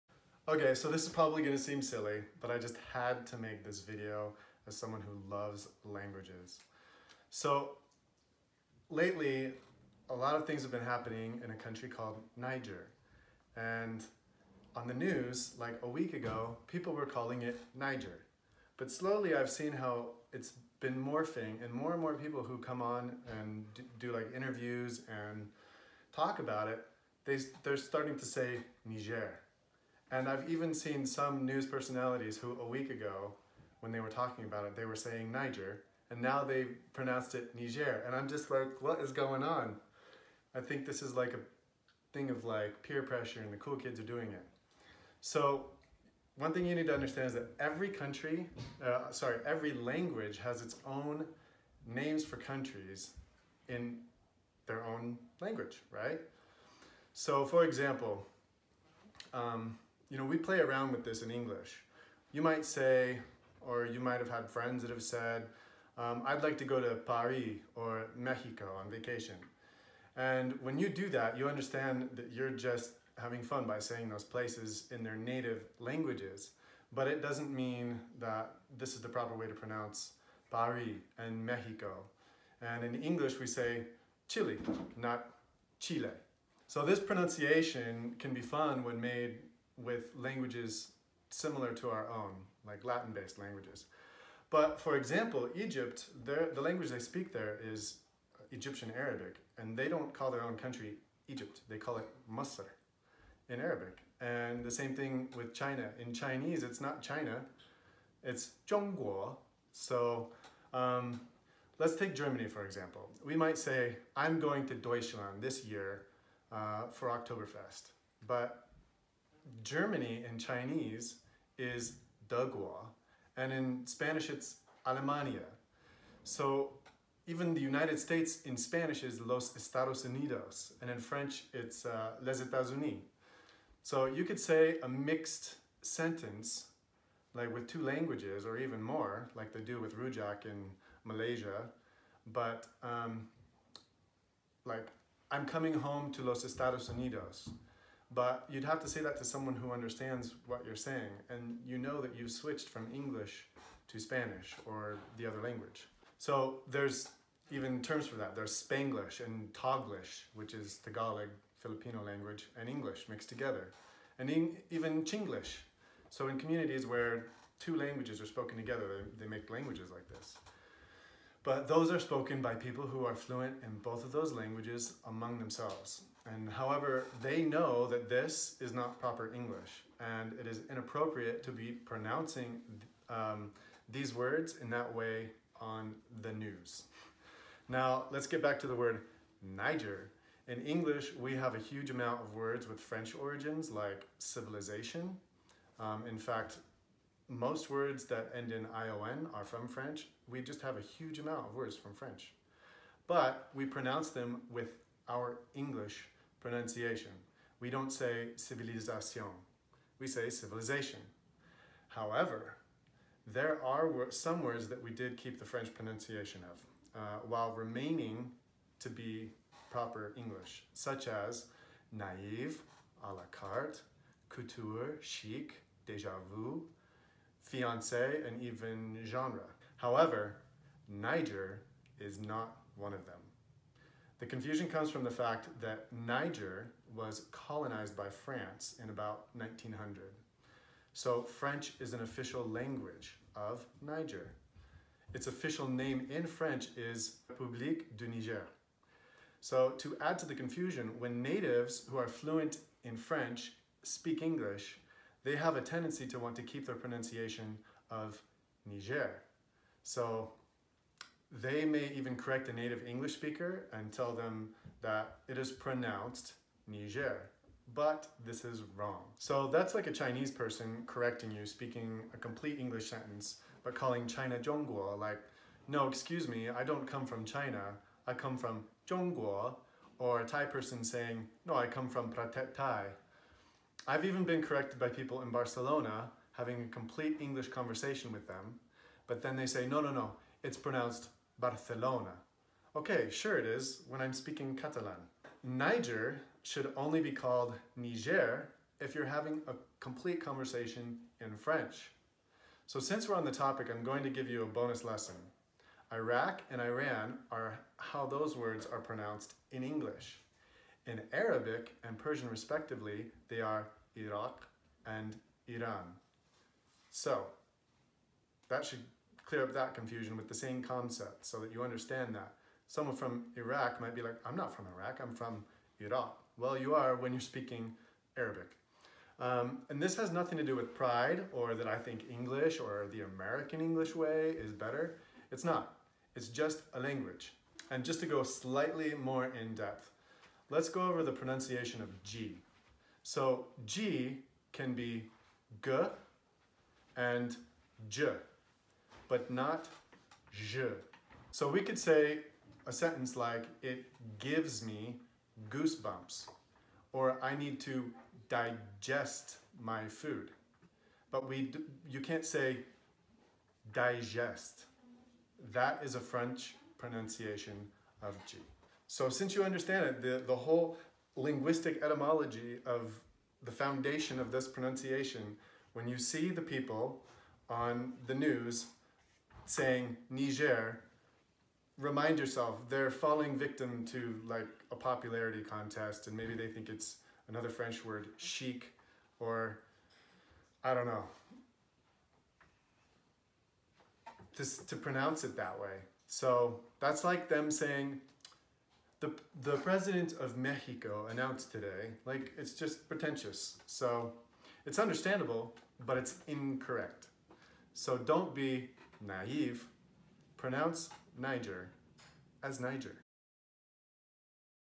How to pronounce Niger (The country), Iraq and Iran in American English.
The proper way to pronounce Niger. The country of Niger. Republic of Niger. How to say Niger the right way in AMERICAN ENGLISH. If you have a different English accent then there is no basis to make an argument against each other on.

Before you comment, please educate yourself about exonyms, endonyms and transliteration. I talked about it in the video but many people seem to not grasp the concept or refuse to.

As a linguist I understand that it can be difficult to differentiate pronunciation, accent, exonyms, endonyms and transliteration, especially when words of different languages are using the same letters to spell it, but pronounce it differently.  This is especially common with the French/English dynamic. But as I state in the video, there are some words in English that retain their French pronunciations. Niger however is not one of them.